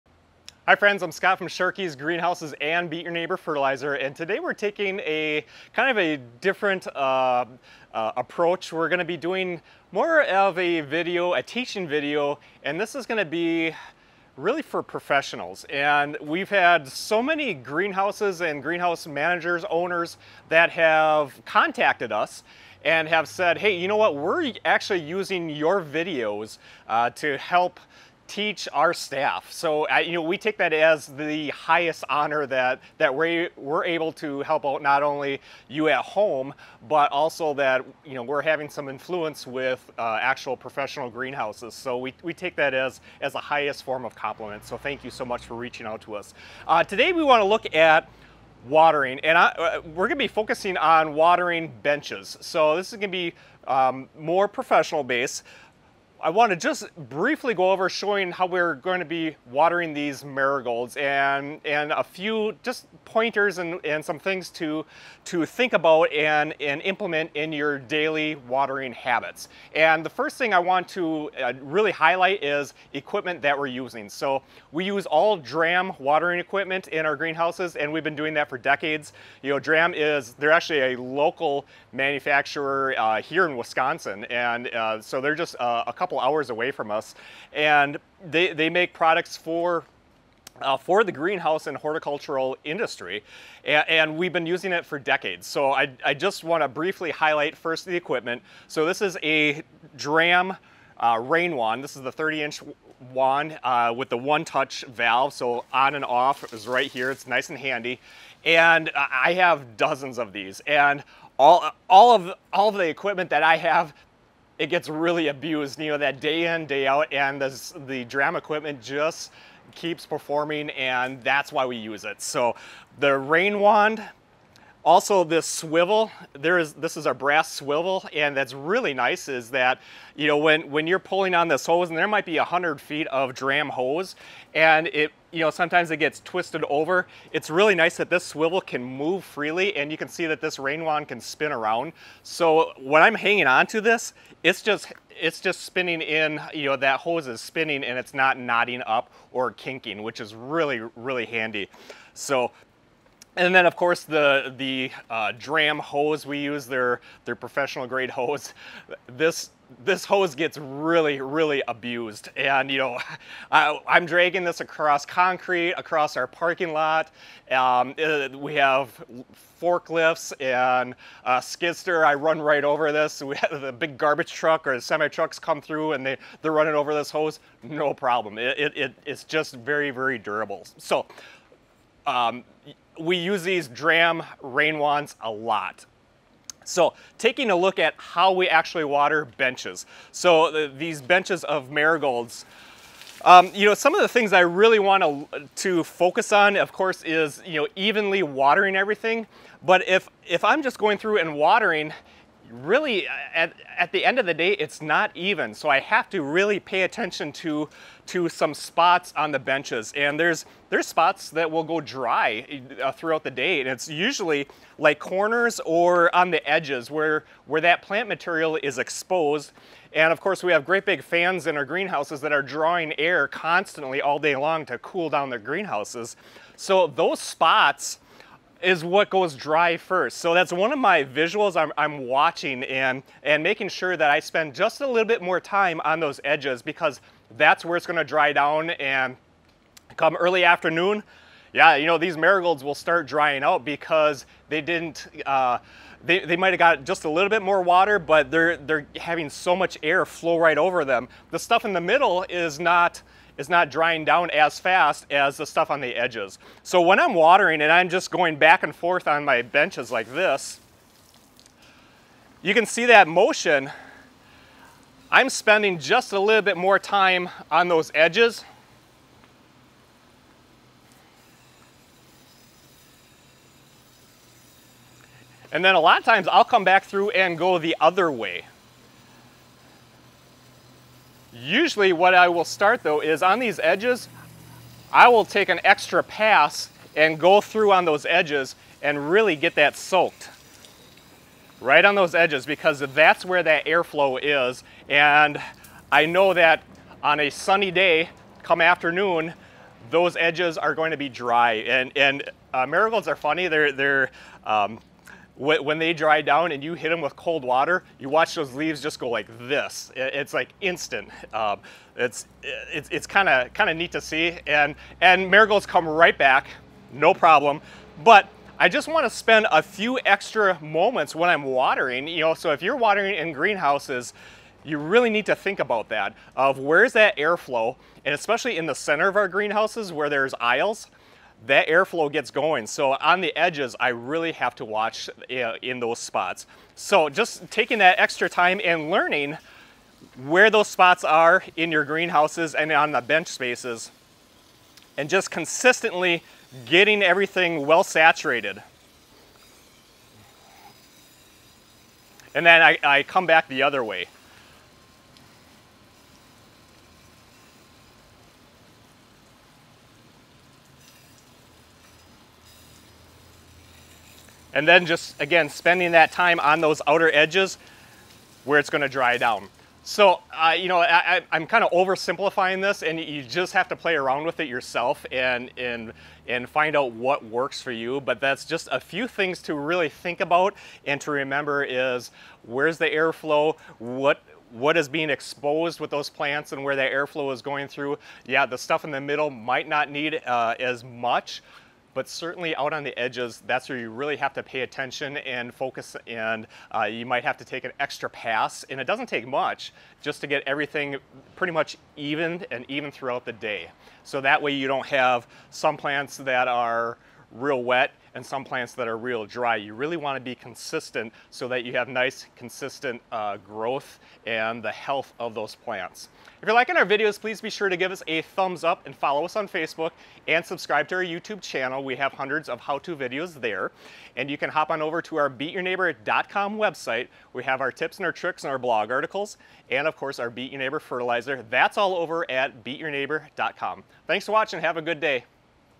Hand WATERING Method in the Greenhouse - How We Water Benches
We show how we hand water in our greenhouses and some of the things that we look for to make sure that there is even coverage. Use these tips for your own professional garden center or home greenhouse.
Dram Rain Wand
Dramm Hose
Dramm Brass Swivel
We have hundreds of great videos on our channel. Be sure to check them out!